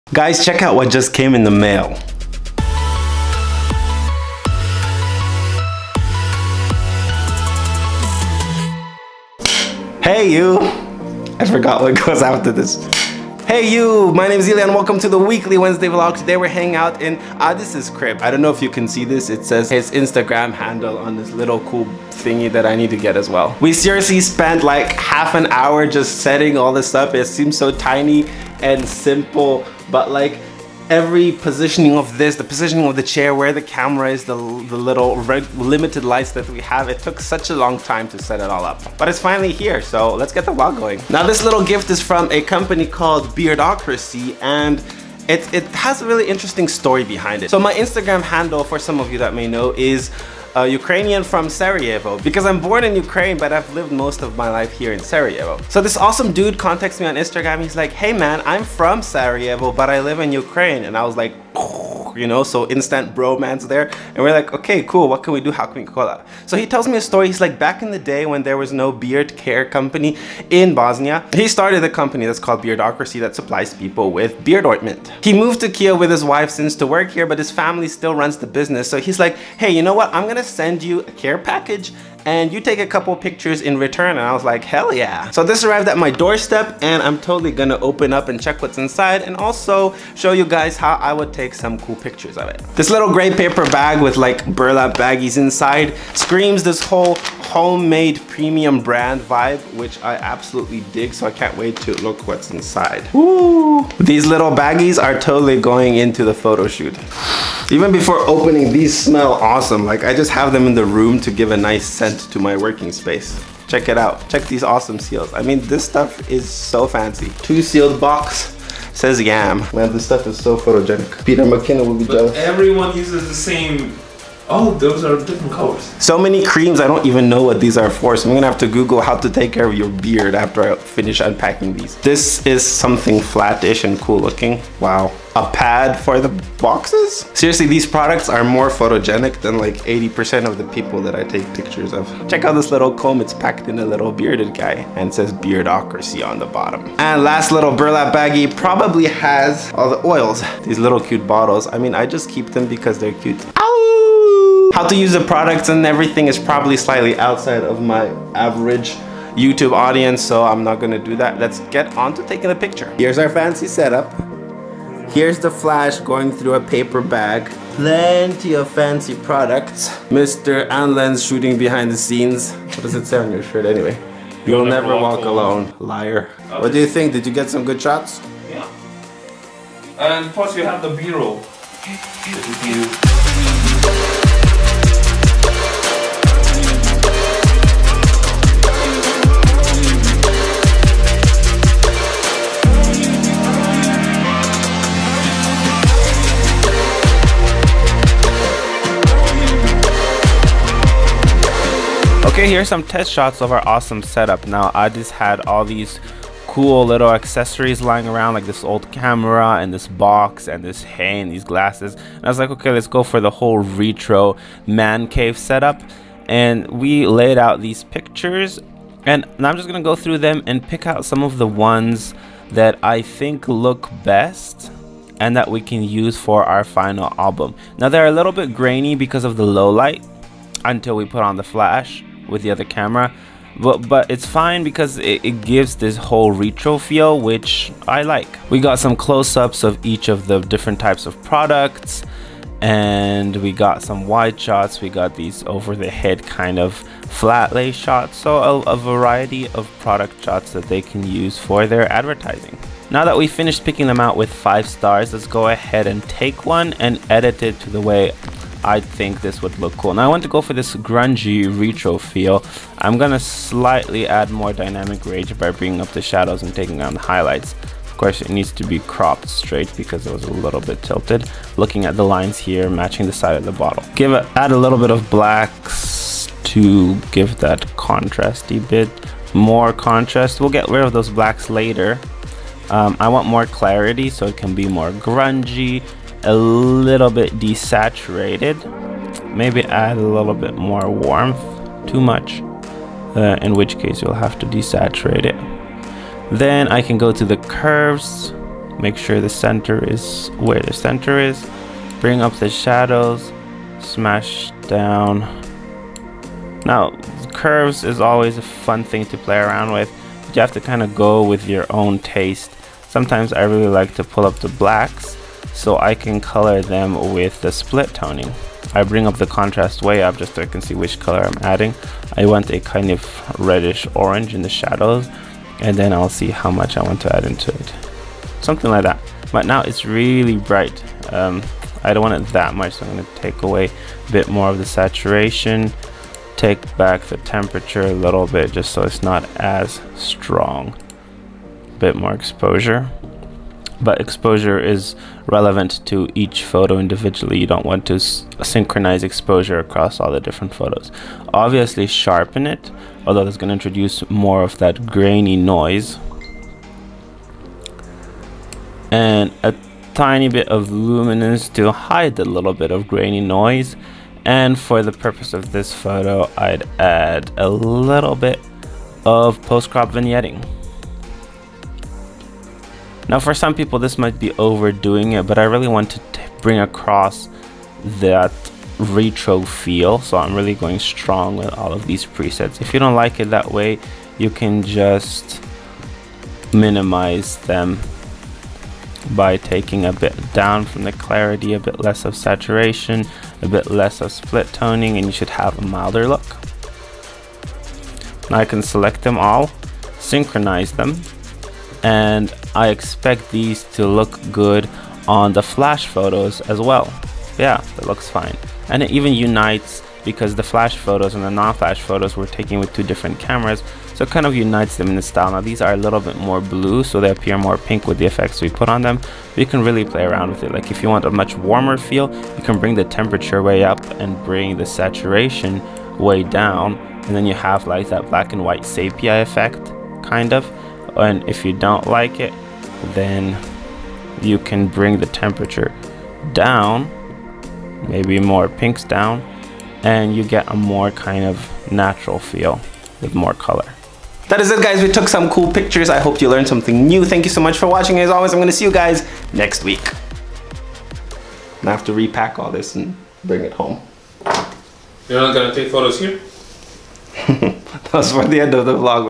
Beard care product photography | Beardocracy
A beard care product package arrived at my door - here's the story behind it, as well as the BTS of the product shots I took of them. What do you think?

My stuff:

18-105mm f3.5-5.6 (very adaptable)
50mm f1.4 (killer lens)